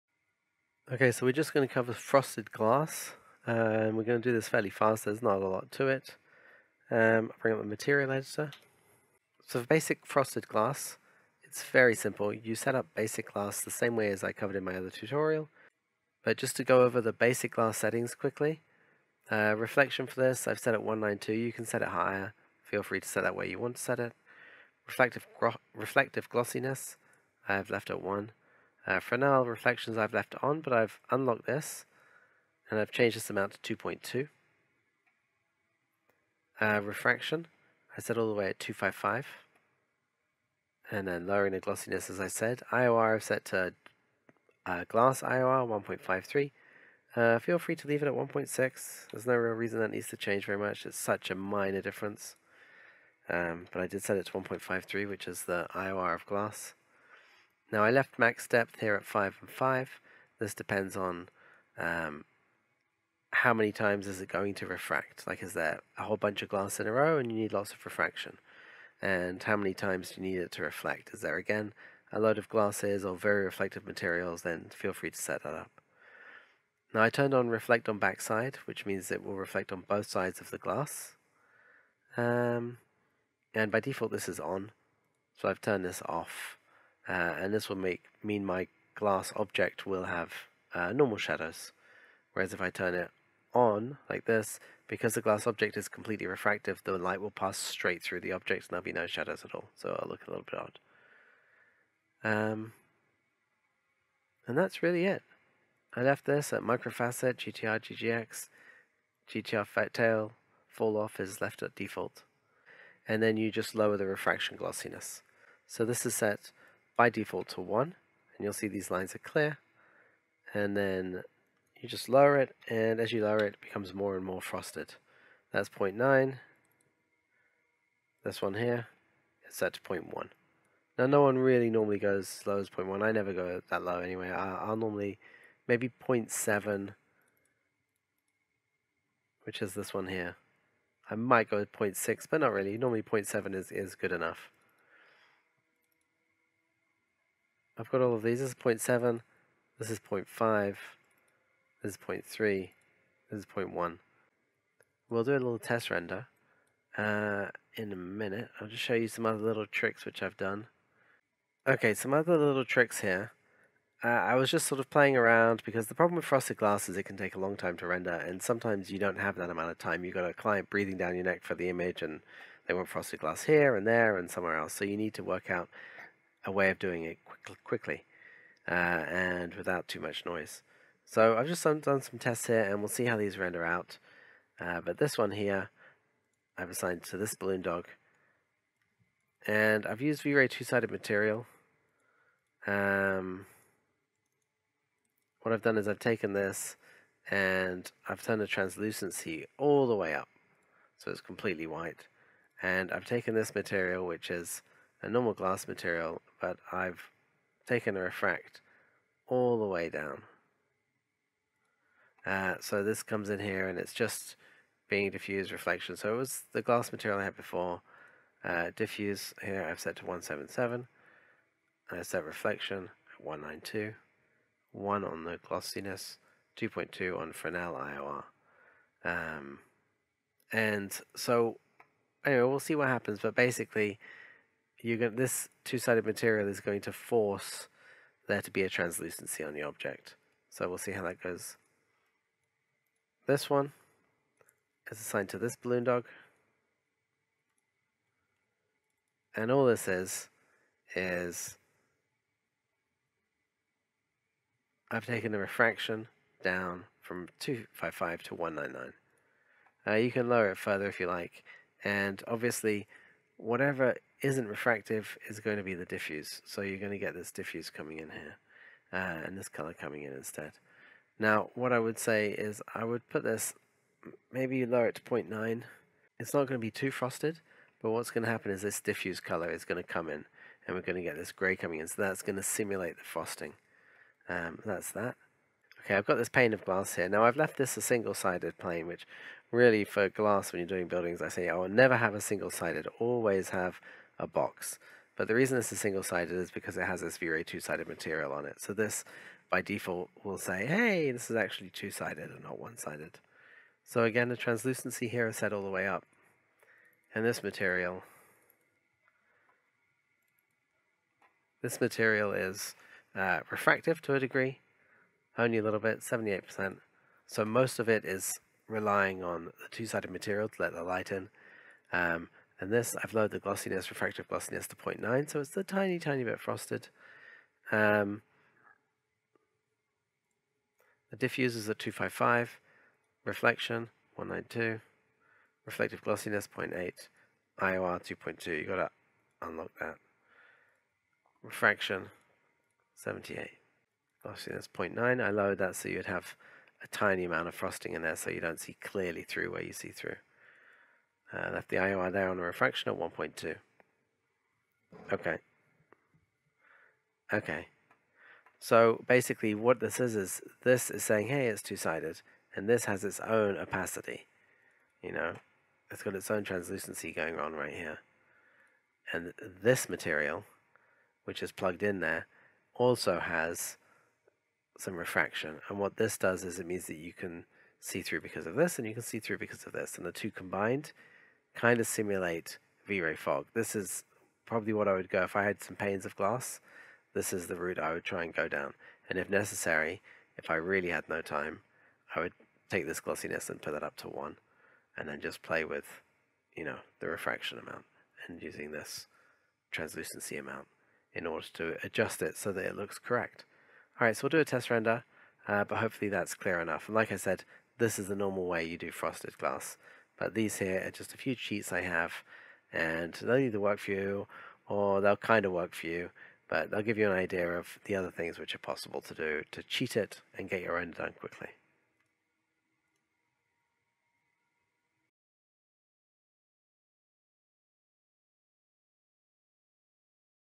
How to Create Photorealistic Frosted Glass in 3DS Max with V-Ray – Archviz & CGI Tutorial
In this tutorial, I show you how to create photorealistic frosted glass in 3DS Max using V-Ray, ideal for architectural visualization (archviz) and CGI projects. Frosted glass is simple to make: set your material to refractive, adjust the reflection and glossiness, and then lower the refractive glossiness to achieve the frosted effect.

I also reference my previous glass tutorial, which covers the basics of creating coloured and clear glass:

This tutorial is part of a series on V-Ray glass materials, which also includes:

Using V-Ray fog for volumetric glass effects
Creating V-Ray glass for windows, with slightly different settings

Key concepts covered:

Creating frosted glass materials in 3DS Max with V-Ray
Adjusting reflection and refractive glossiness for realism
Preparing glass materials for archviz and CGI scenes
Following a workflow for consistent photorealistic glass results